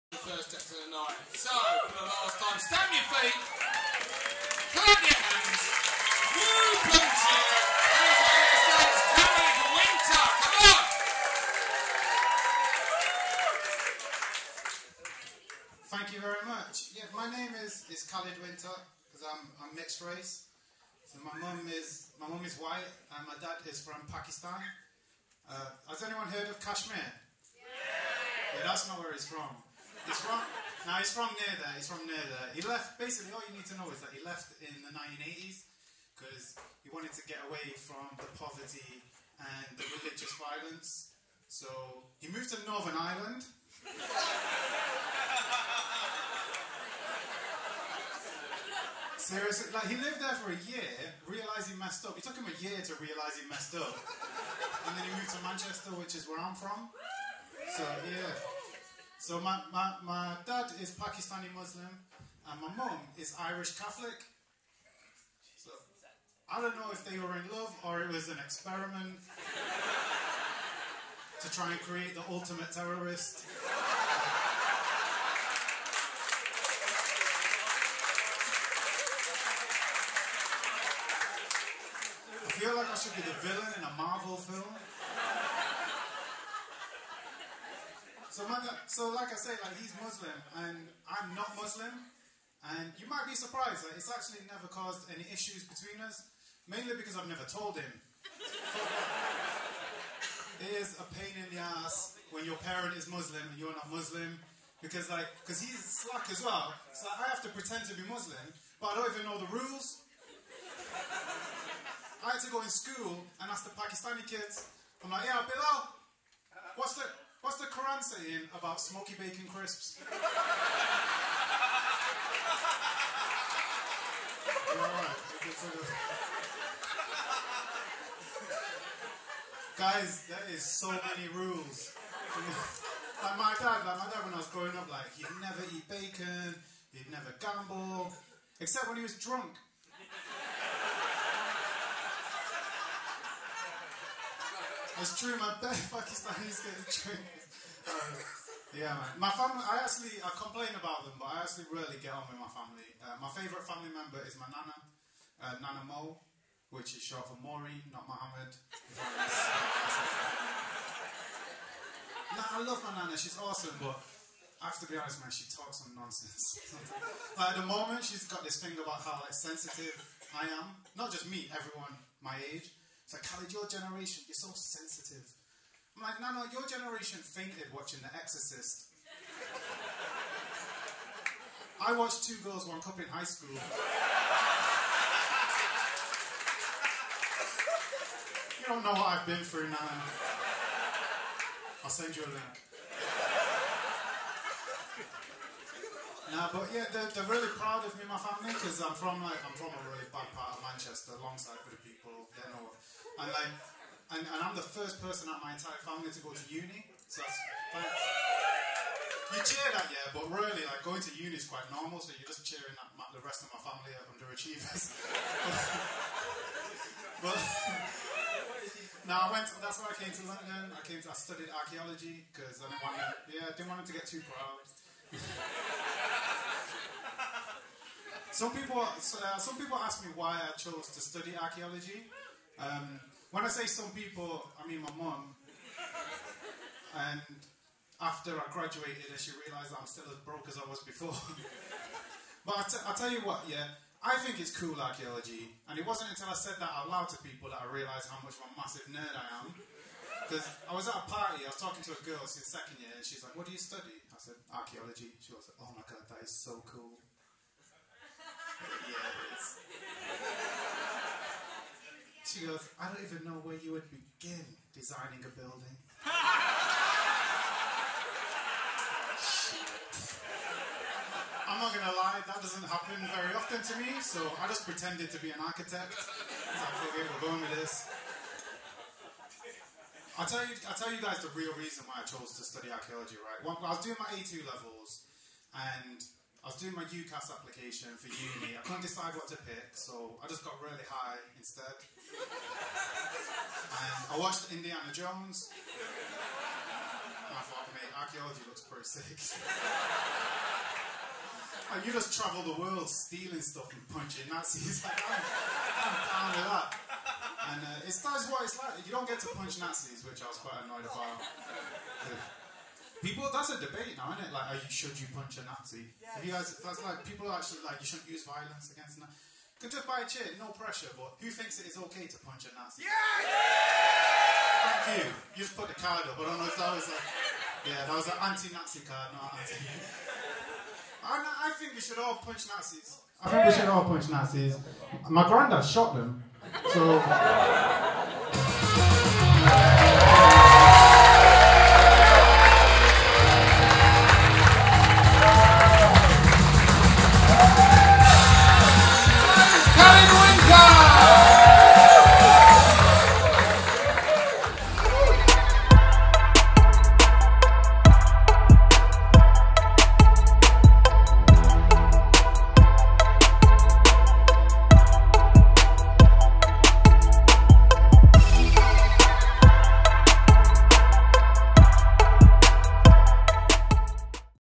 The Blackout - PRETEND TO BE MUSLIM - Khalid Winter Comedian - Stand Up Comedy - Funny
Khalid Winter Comedian